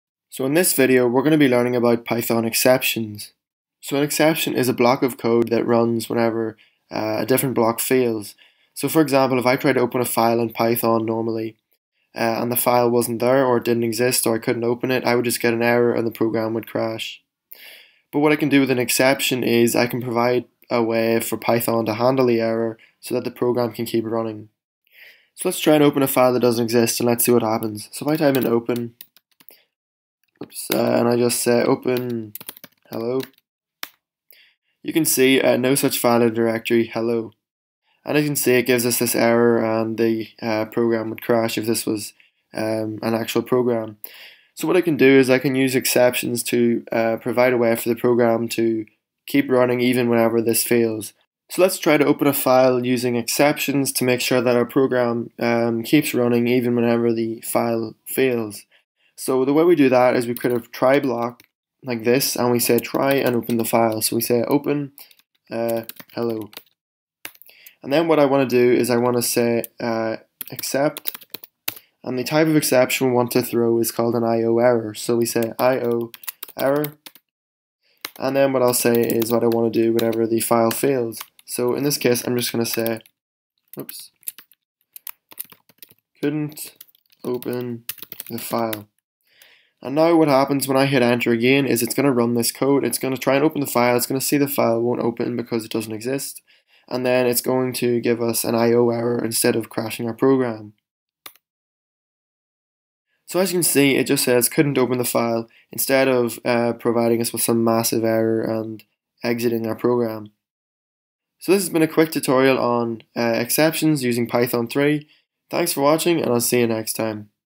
Python 101 - Python Exceptions
In this video we will be learning about Python Exceptions and how to use them to prevent our programs from crashing.